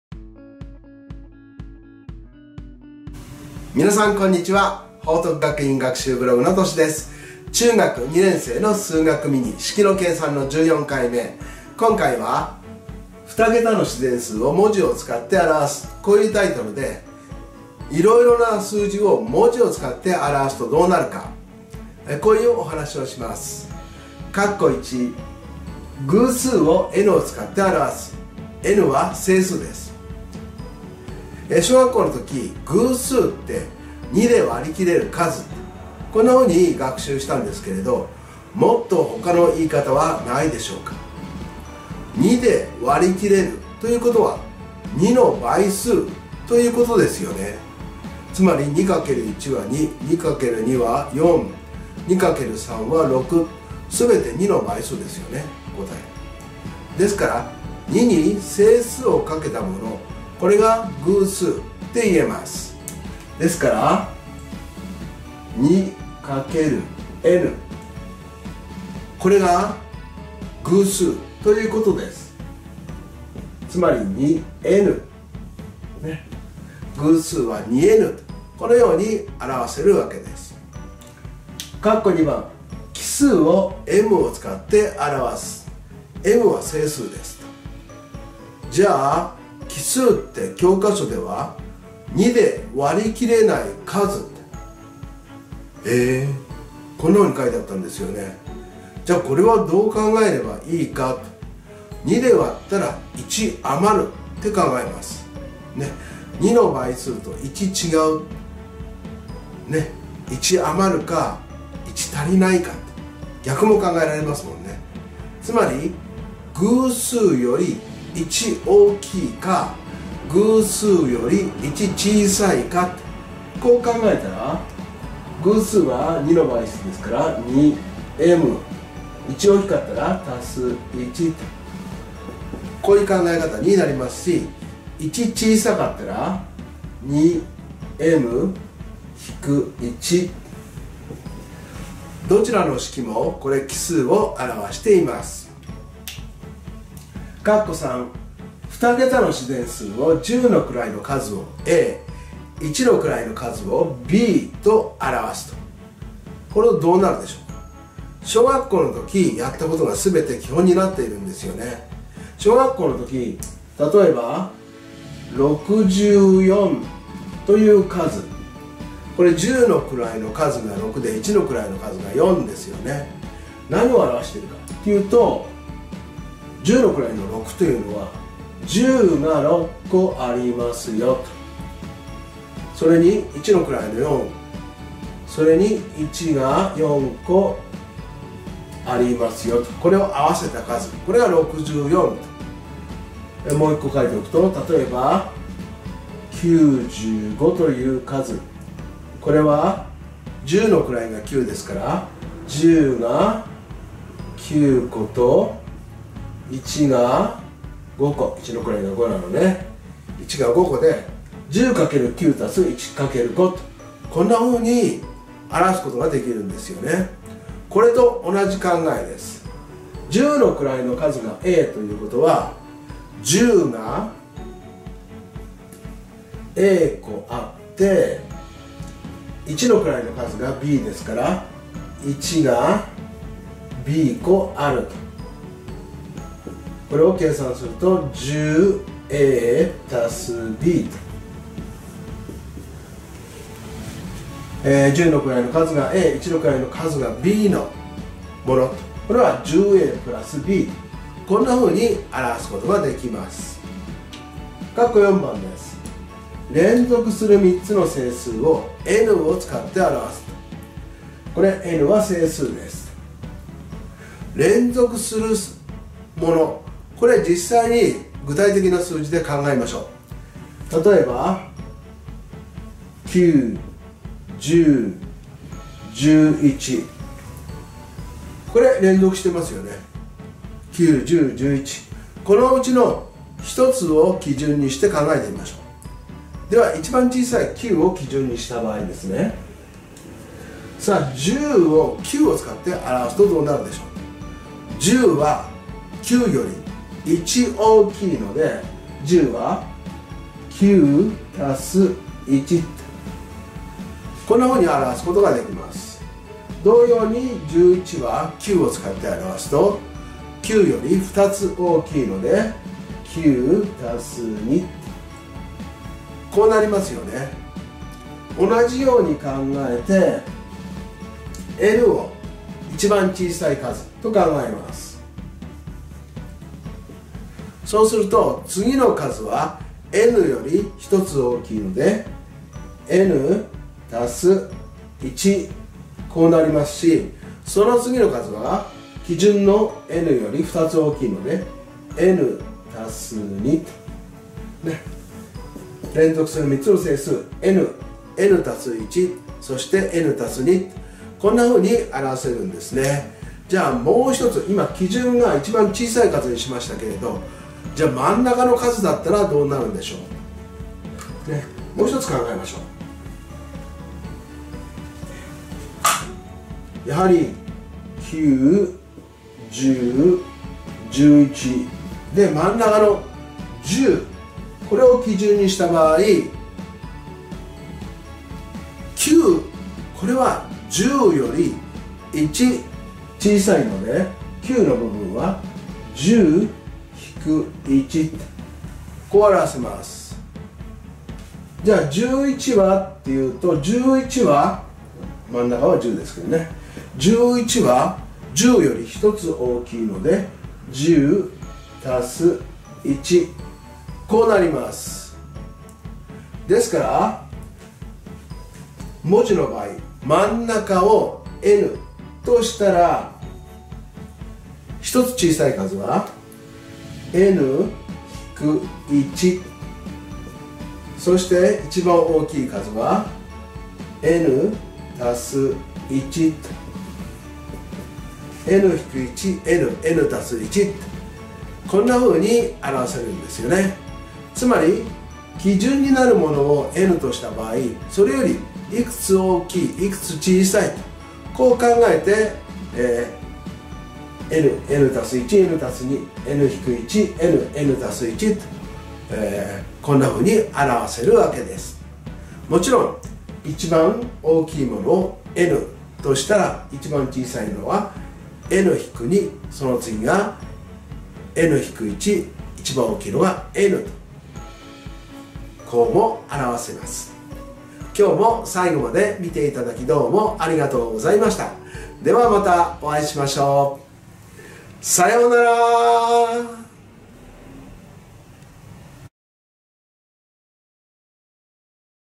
2桁の自然数を文字を使って表す 中2数学ミニ 式の計算14
2桁の自然数を文字を使って表すとどうなるか？偶数や奇数は、さらに3つの連続する整数、考え方を覚えましょう。